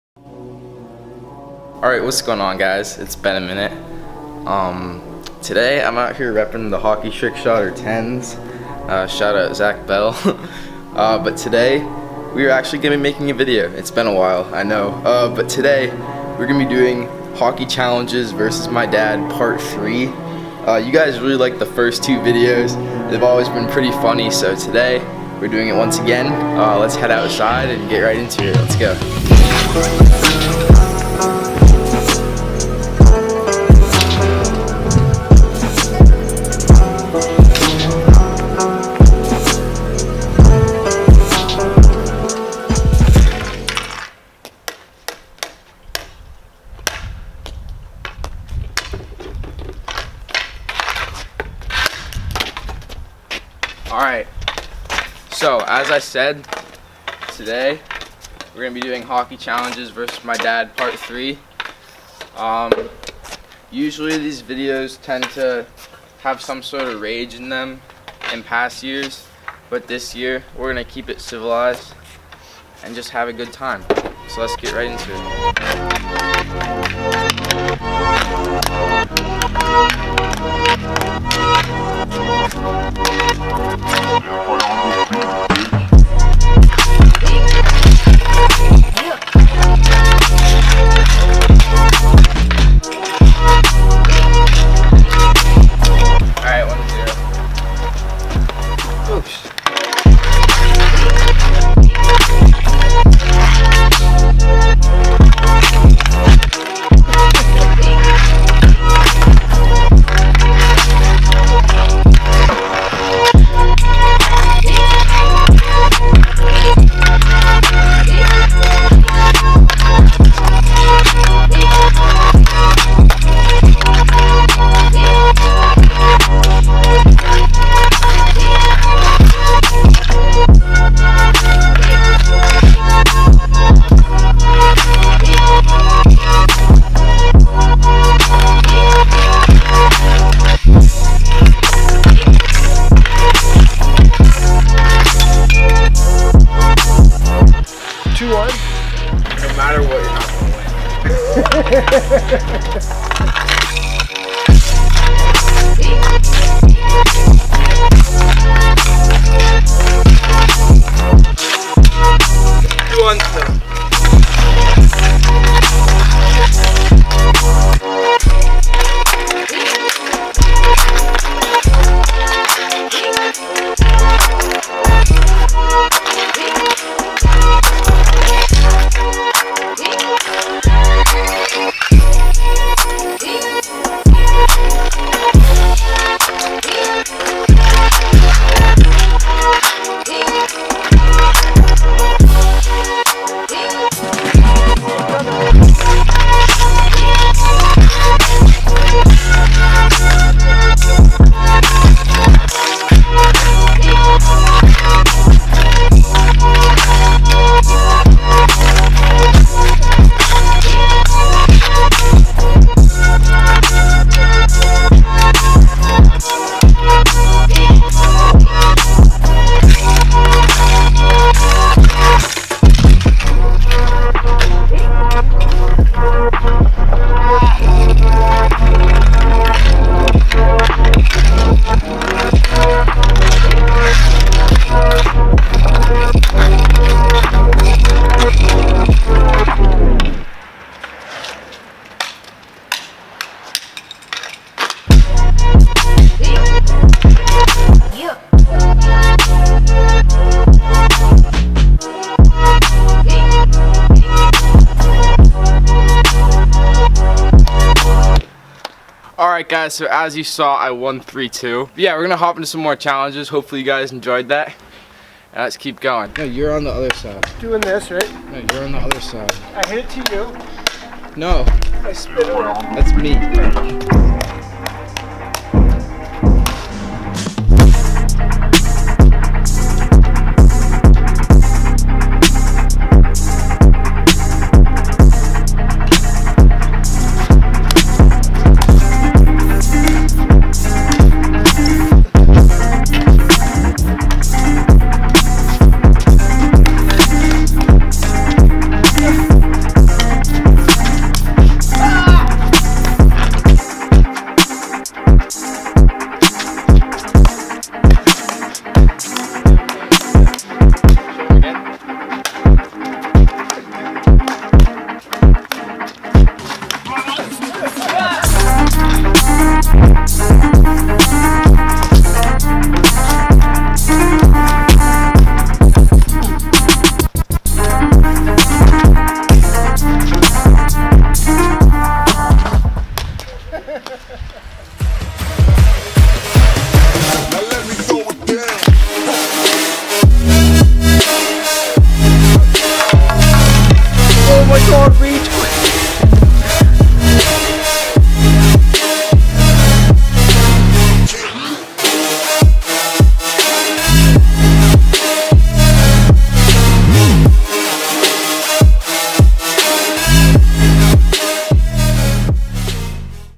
HOCKEY CHALLENGES VS MY DAD! (PART 3)
Hey guys, I'm back... again. This video turned out really well, I hope you enjoy it :) Thanks for sticking around, many more videos to come. Winter grind soon!!